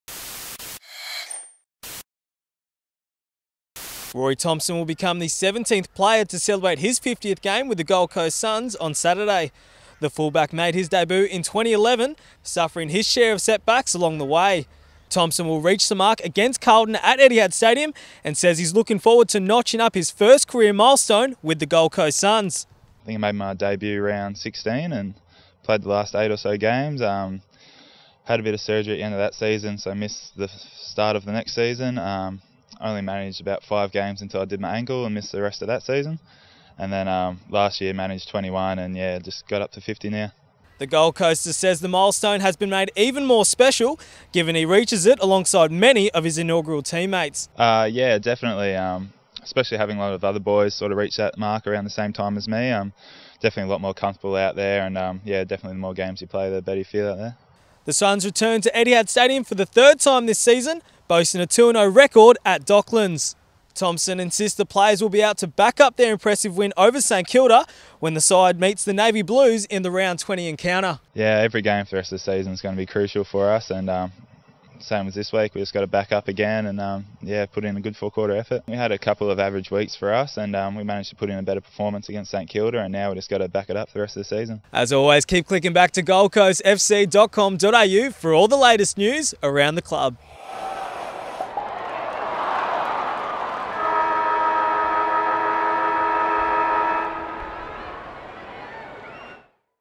Thompson to reach 50 game milestone
GC SUNS defender Rory Thompson will run out for his 50th game for the club when the SUNS take on Carlton at Etihad this weekend.